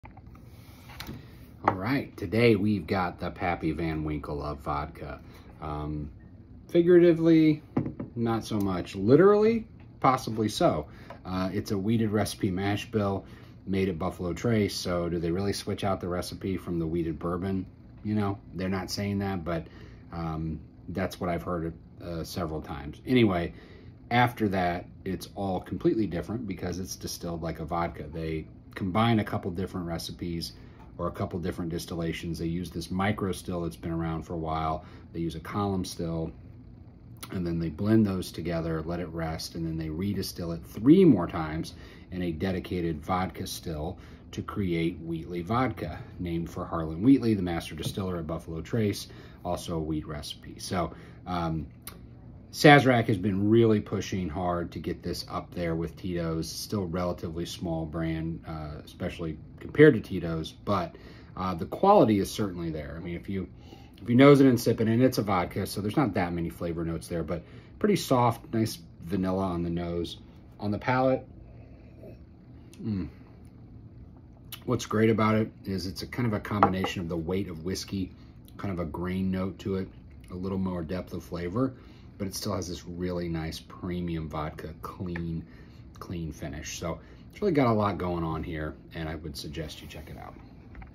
Wheatley Vodka
Likely produced from the same wheated mashbill as Weller and Pappy, Harlen Wheatley's own "Wheatley" vodka is unique in the vodka world, a blend of micro-distilled vodka and column distilled vodka, then married and distilled 3 more times in a dedicated vodka still, for a total of 10 unique distillations. The nose is pillowy soft vanilla, while the palate has the perfect amount of grainy weight with the very smooth and clean finish you'd expect from a super premium vodka. If you're a Tito's fan (and we are!) you may want to also give Wheatley's a shot, it's more of an on the rocks sipper. 🥃🍸👏 The Pappy of Vodka!